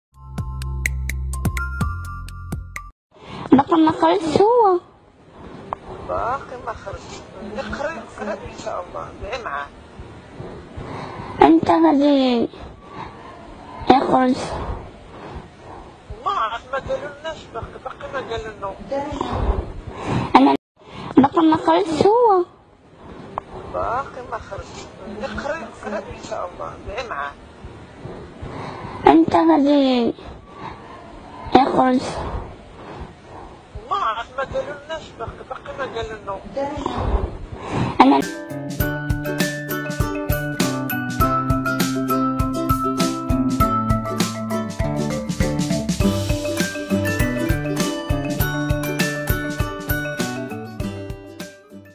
عااااجل نزهة الركراكي تتحدث مع الطفلة آية و تقول: "سعد لمجرد قريب يخرج" - Saad Lamjarred
عااااجل نزهة الركراكي تتحدث مع الطفلة آية و تقول: "سعد لمجرد قريب يخرج"
قضية
فرنسا
بشير عبدو
لورا بريول